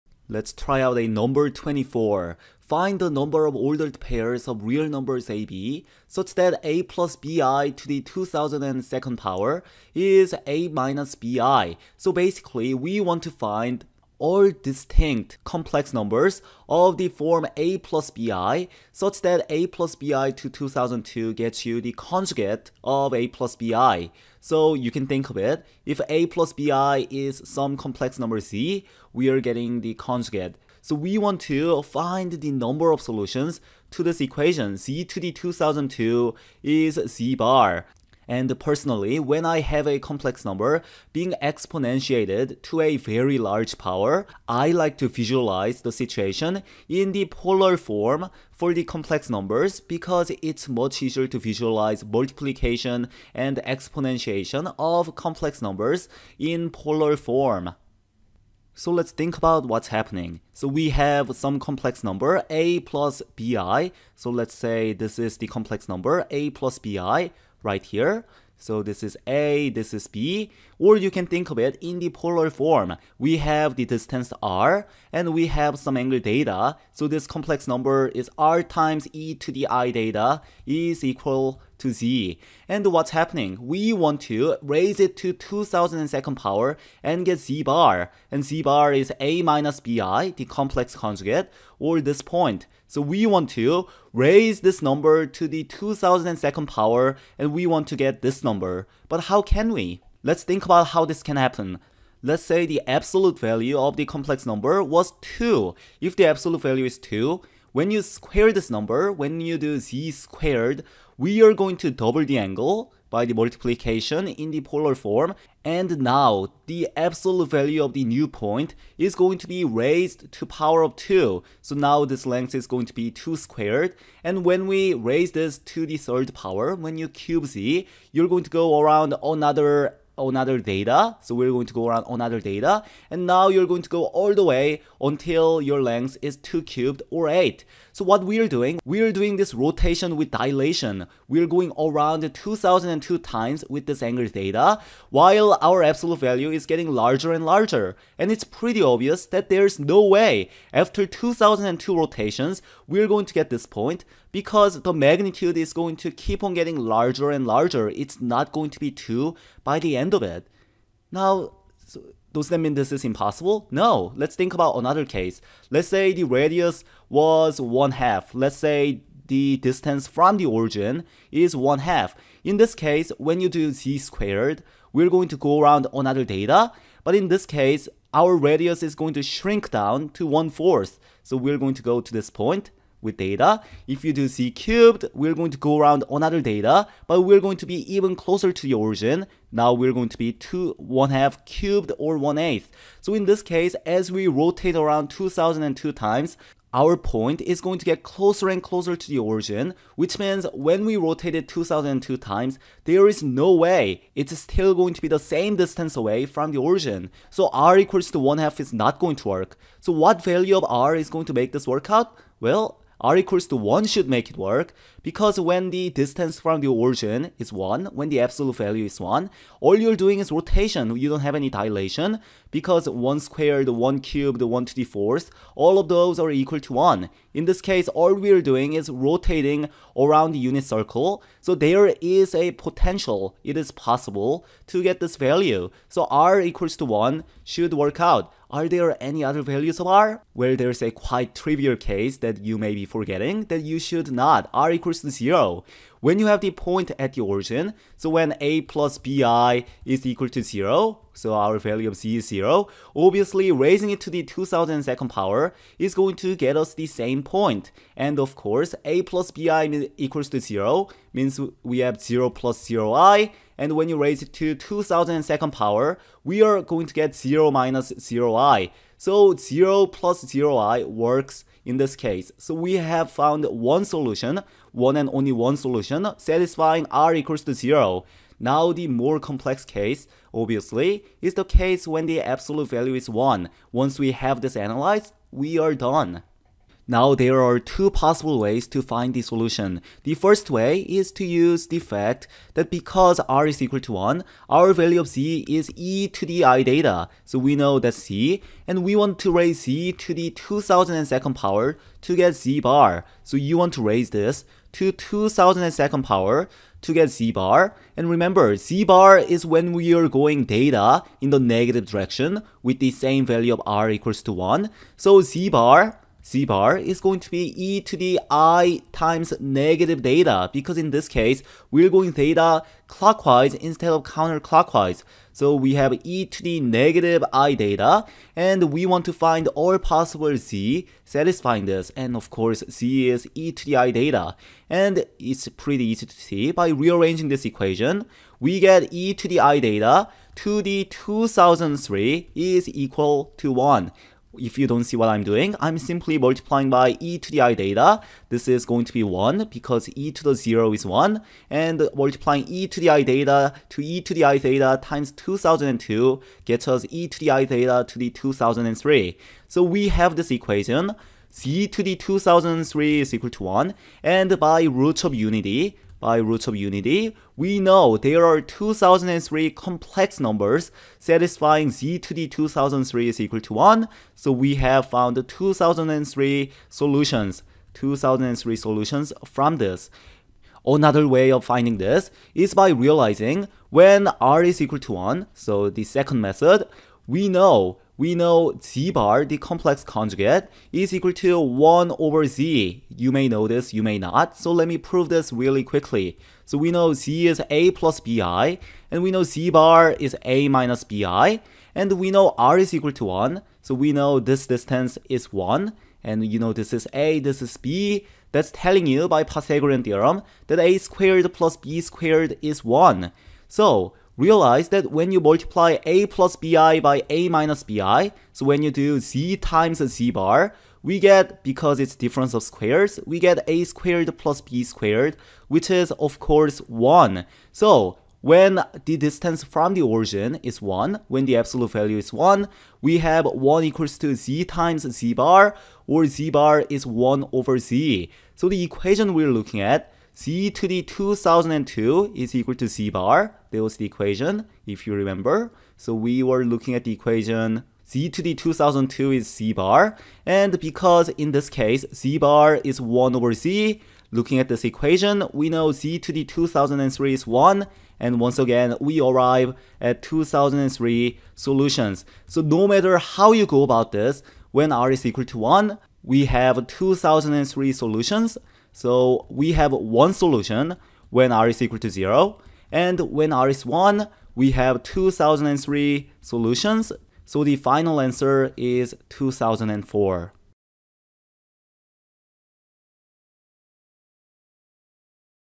AMC 12: How Many Complex Solutions?? (2002 A #24)
Let's rotate our way around the complex plane and see if we land on the conjugate.